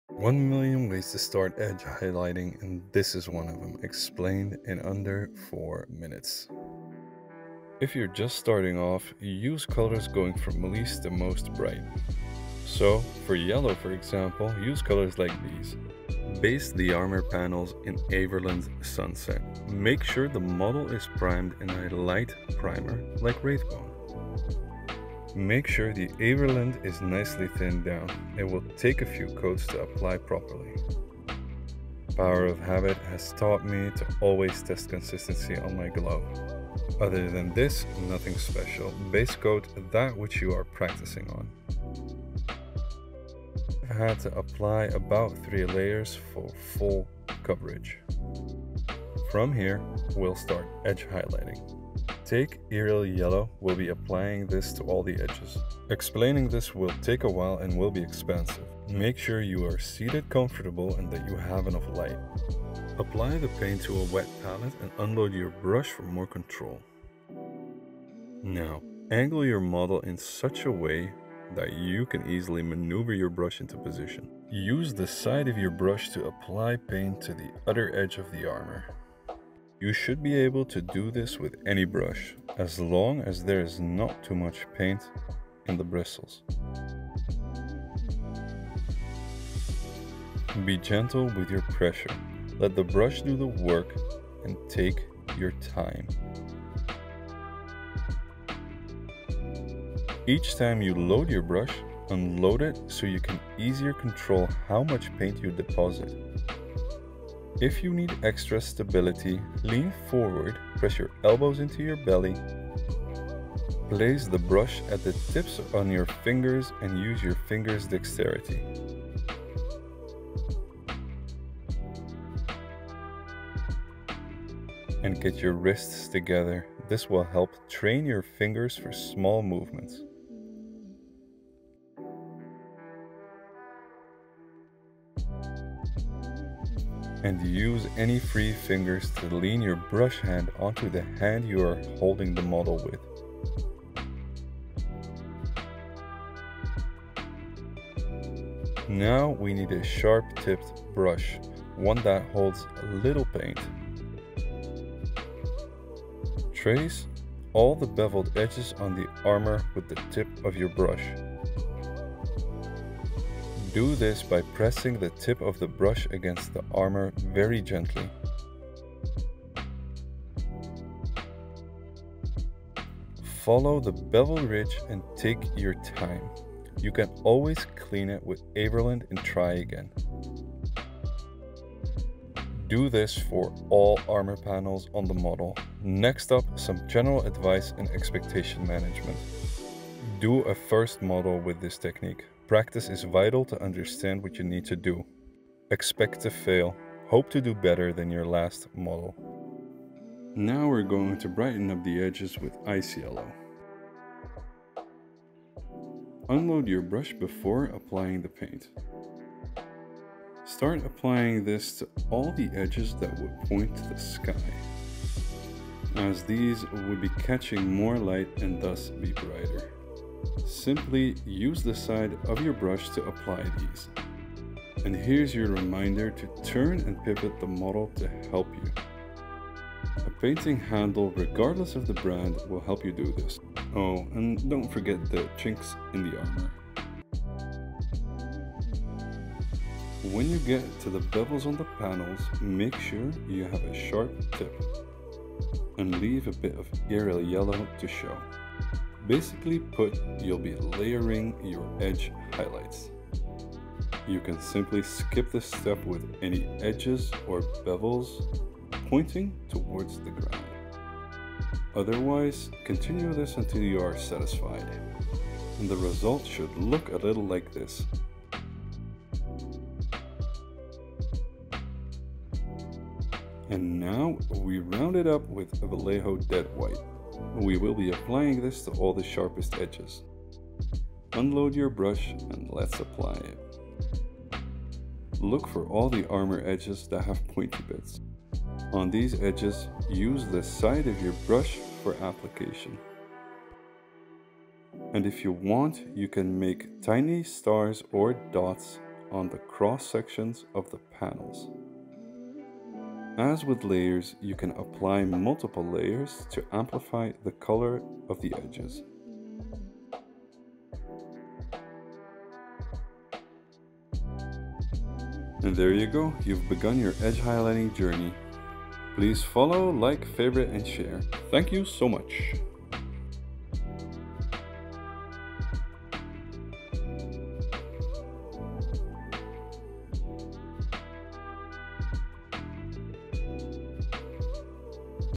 Edge Highlighting Warhammer Miniatures - Easy to follow tutorial
In this tutorial we focus on how to edge highlight Warhammer 40k miniatures. The techniques used can be applied to any miniatures and helps to add a little more depth and visual interest to your finished pieces.

My aim is to help you feel confident when painting your Warhammer 40k miniature figures and to help you enjoy the process.